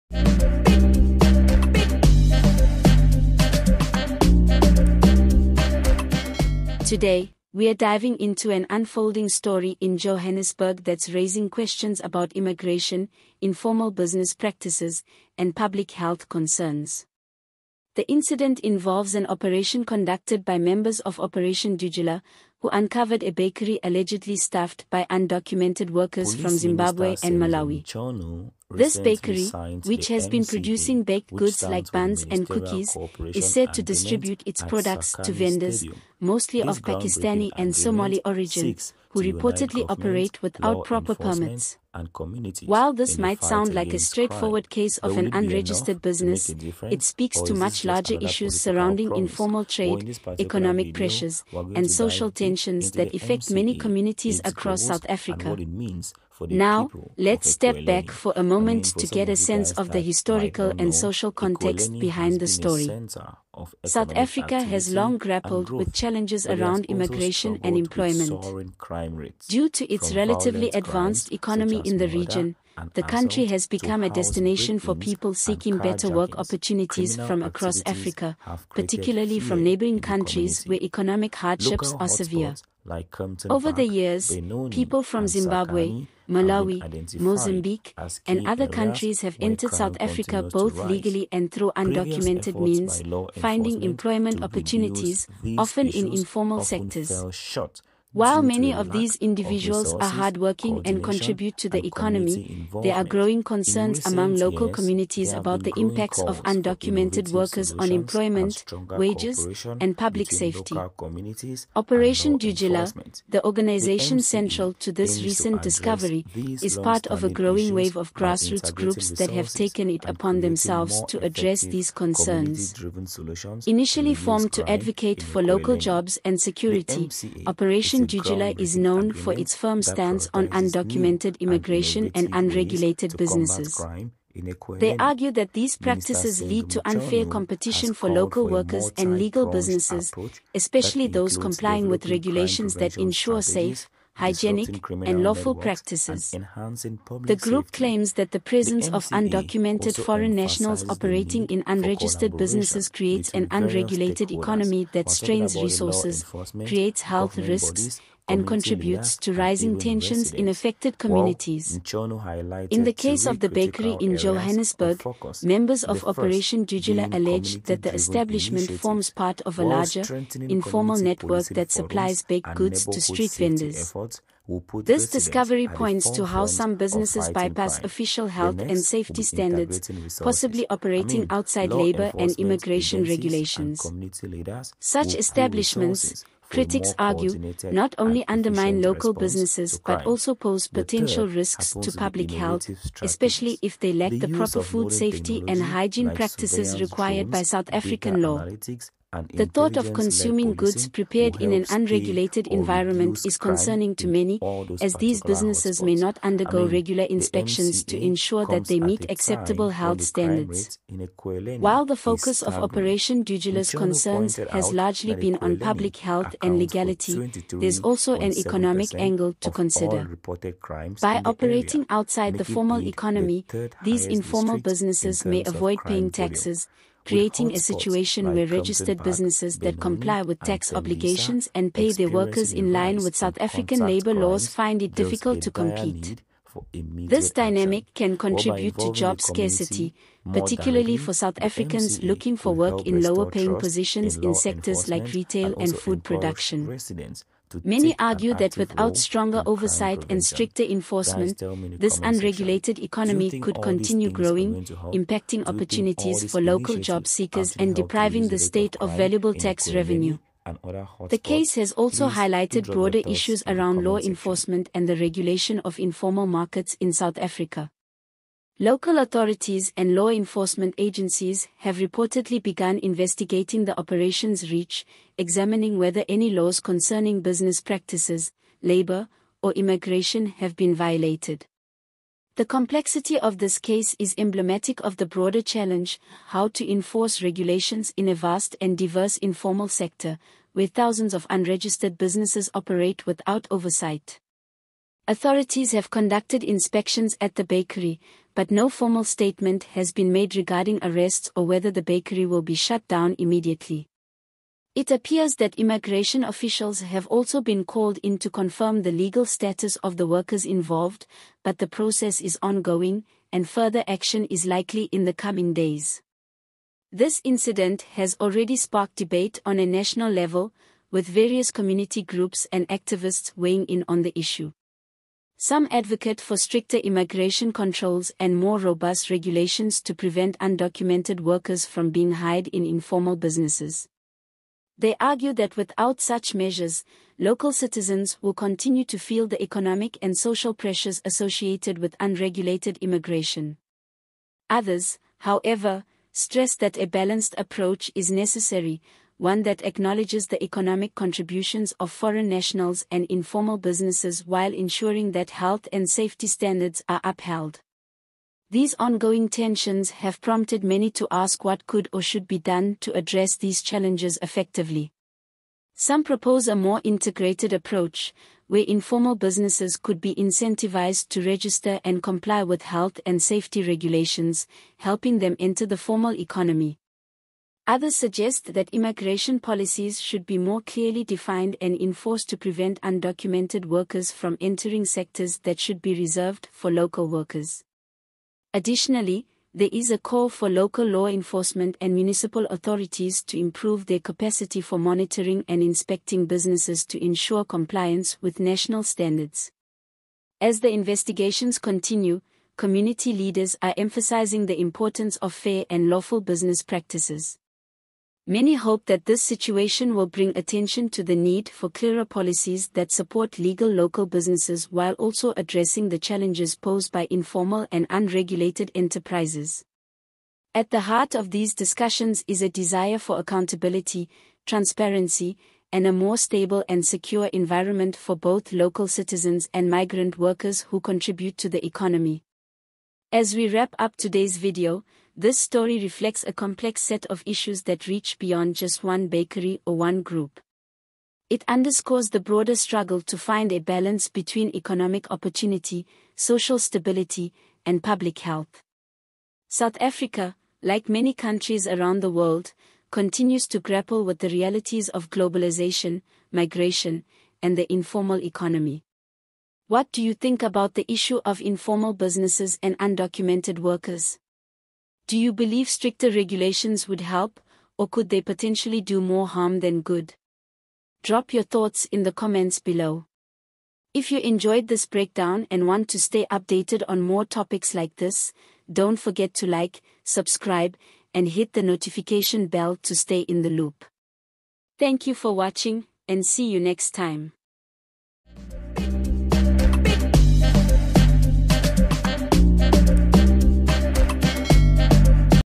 SAPS Officers in Shock after discovering A Camp Run By illegal Foreigners. This is very painful 😳
In today’s video, we explore a developing story in Johannesburg that has raised significant questions about undocumented immigration, public health, and the informal economy. Members of Operation Dudula recently uncovered an unregistered bakery allegedly run by undocumented workers from Zimbabwe and Malawi. The bakery’s products, mainly buns and cookies, are said to be distributed to local vendors, sparking discussions about unregulated business practices and their potential risks. This case sheds light on the complexities surrounding informal trade, economic pressures, and social tensions within South Africa.

The story serves as a lens into broader issues related to undocumented labor, health and safety in informal businesses, and the challenges facing local entrepreneurs in a competitive market. Authorities have begun investigations, but the situation highlights the need for balanced regulations that protect both public health and local job opportunities. This video dives into the impacts and concerns raised by unregistered businesses, as well as the viewpoints of different community groups on how to address these issues fairly.

What’s your take on informal trade and the role of undocumented workers in South Africa? Should stricter regulations be imposed, or could a different approach be more effective? Join the conversation in the comments and share your thoughts!

1. Do you think stricter immigration and business regulations could help address issues like this?

2. Should informal businesses be encouraged to register and follow health and safety standards?

3. What impact do you think undocumented workers have on local job markets?

4. How should communities balance the contributions of foreign nationals with the needs of local workers?

5. Would you feel comfortable buying food from informal businesses if health standards were not clearly regulated?